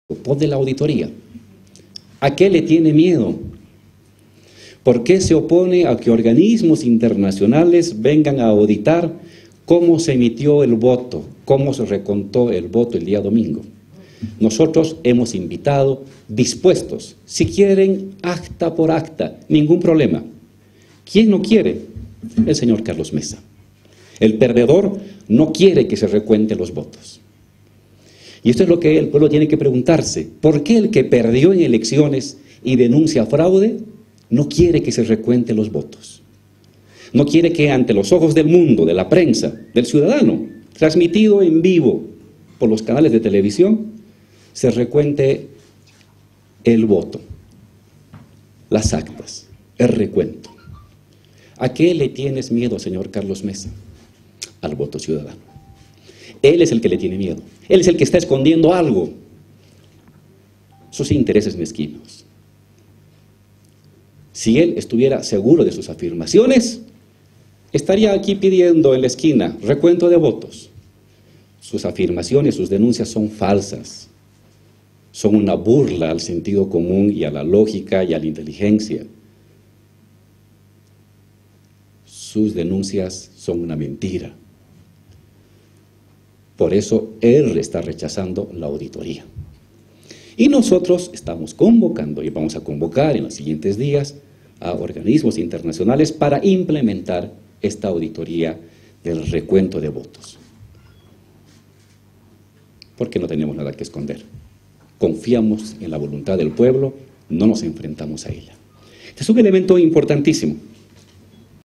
28/10/19 ALVARO GARCIA LINERA- VICE PDTE. BOLIVIA.
ASEGURA QUE CARLOS MESA TIENE TEMOR A LA AUDITORIA QUE REALIZARAN ORGANISMOS INTERNACIONALES PORQUE SABE QUE PERDIÓ EN LAS ELECCIONES.